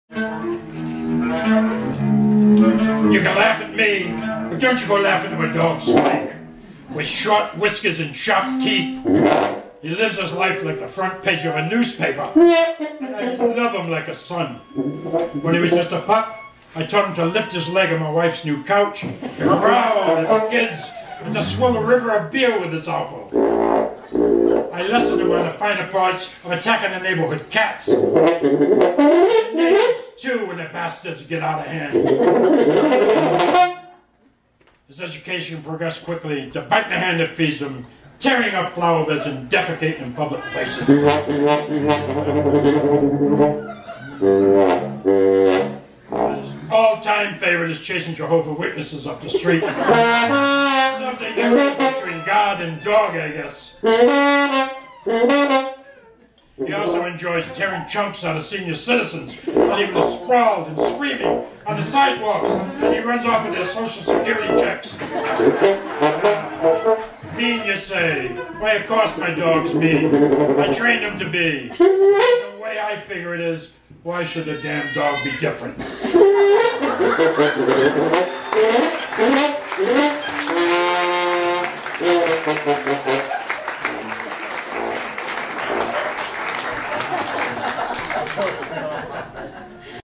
MyDogSpike
Parent-to-Parent Family Support Network of the High Country celebrated
their 25th Silver Anniversary at Holy Cross Episcopal Church with special
guests Earl LeClaire (poet and chef) reads a poem in which he talks about
his dog, Spike. Earl is accompanied by two fellow musicians Harold
McKinney (Trombone) and Ken Lurie (Cello) who are on the faculty at
Appalachian State University, School of Music